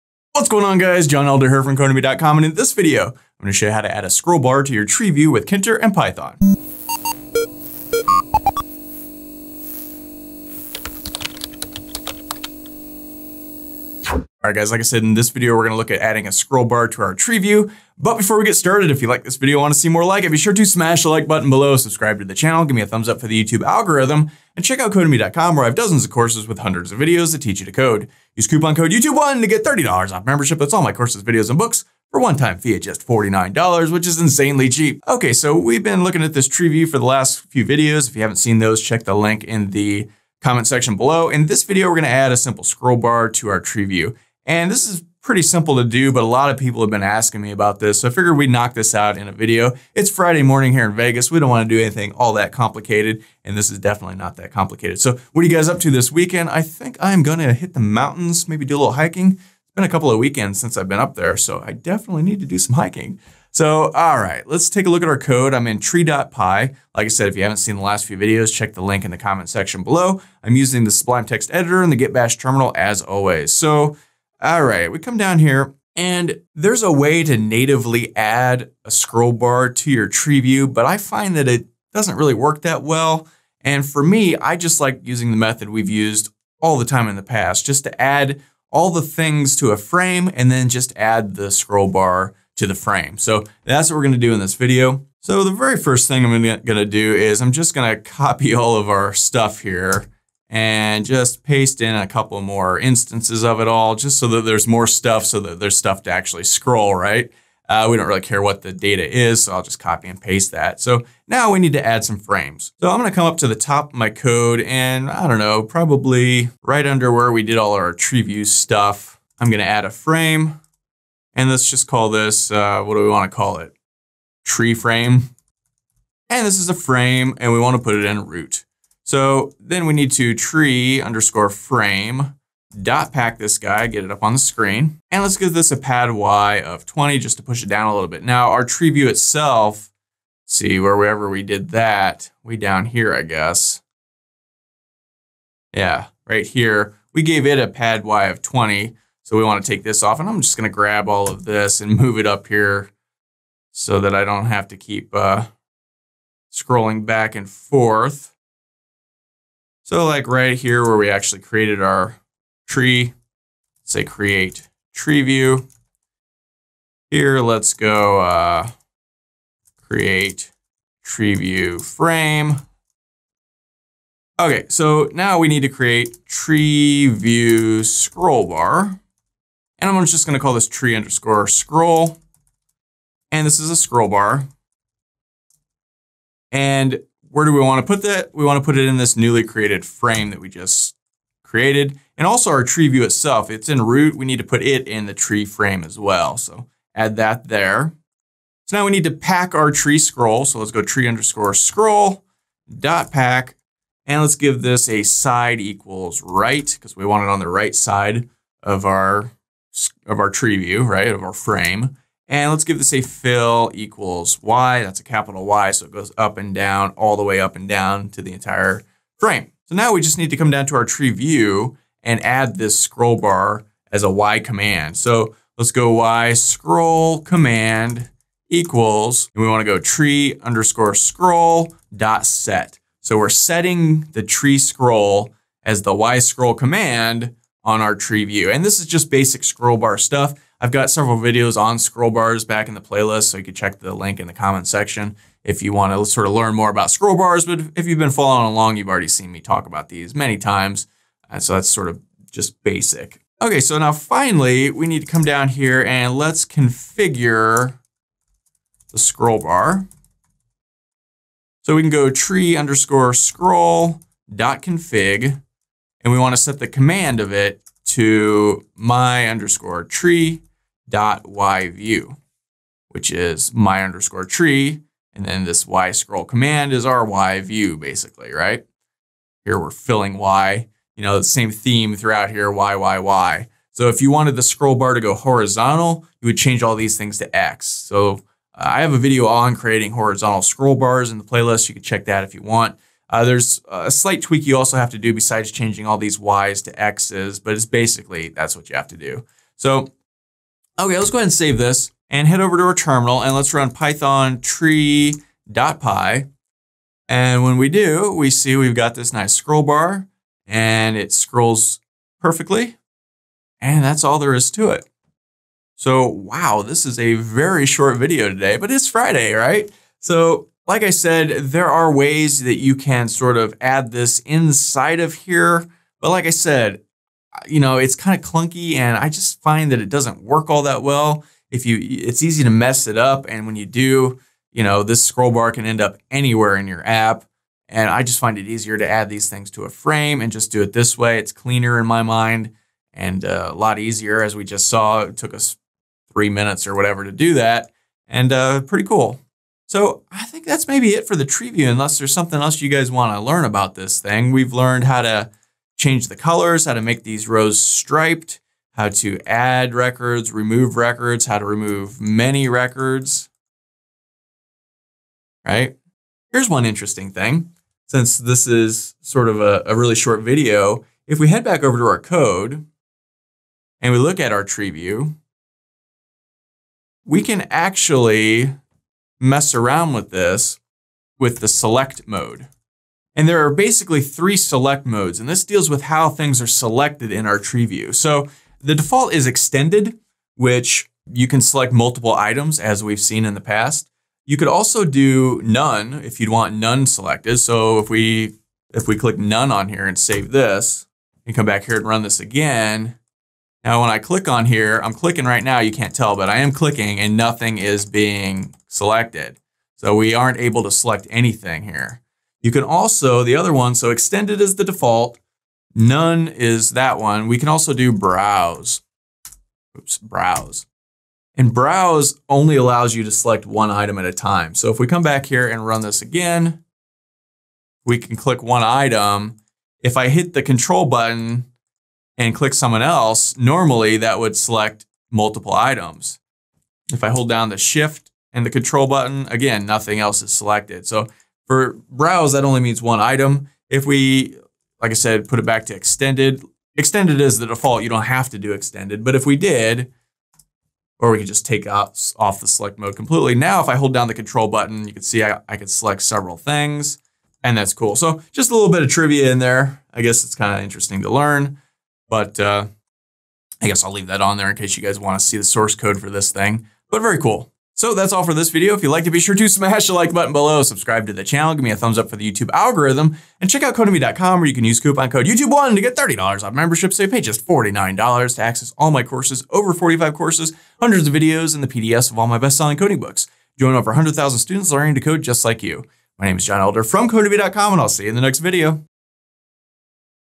Treeview Scrollbar - Python Tkinter GUI Tutorial #120
In this video I'll show you how to add a scrollbar to your treeview widget with tkinter and Python.

Adding a scrollbar to a treeview widget is just like adding any scrollbar to anything.  Treeview has the ability to take a scrollbar, but we won't use that. Instead we'll add our treeview widget to a frame, and then add the scrollbar to the frame as we've done in many videos in the past.

In this video I'll also show you the three treeview selectmodes, extended, browse, and none.  These allow us to determine how many or few rows in the treeview a user can select.